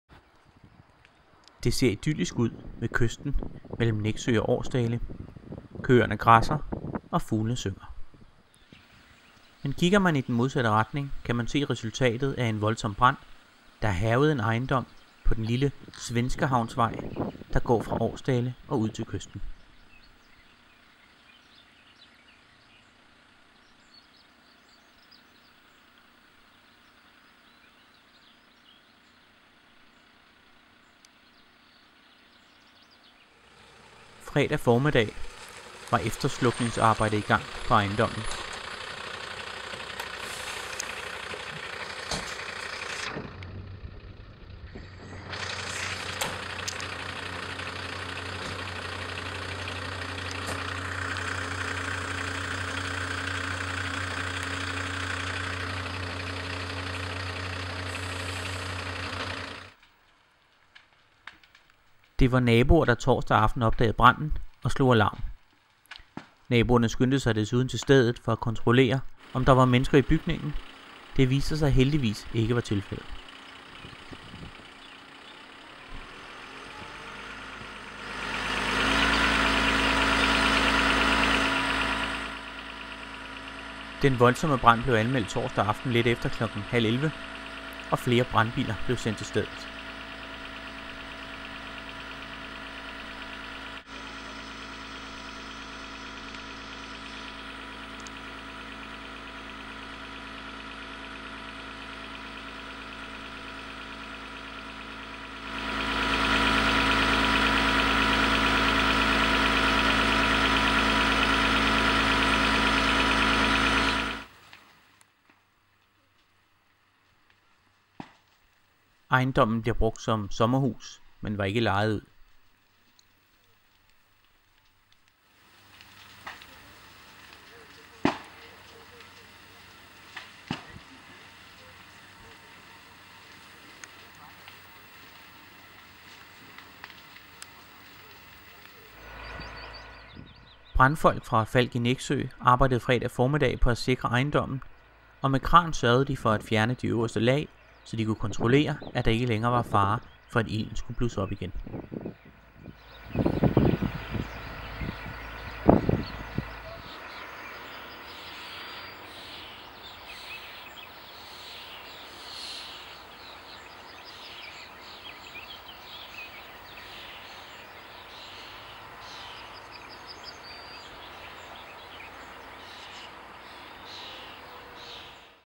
Brand i sommerhus
En voldsom brand hærgede en ejendom mellem Nexø og Aarsdale